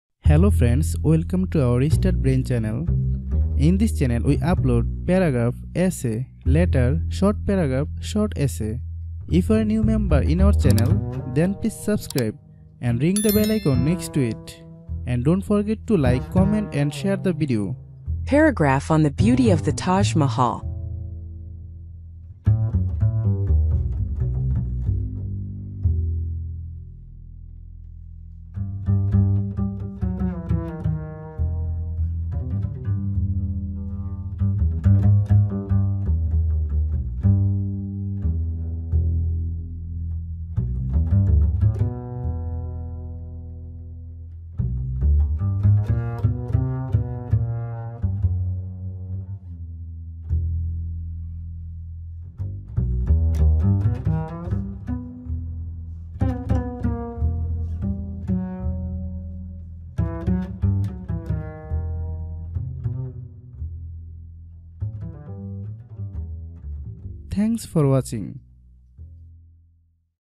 Paragraph on The Beauty of the Taj Mahal
Hello friends,
  In this video I have written a Paragraph on The Beauty of the Taj Mahal in English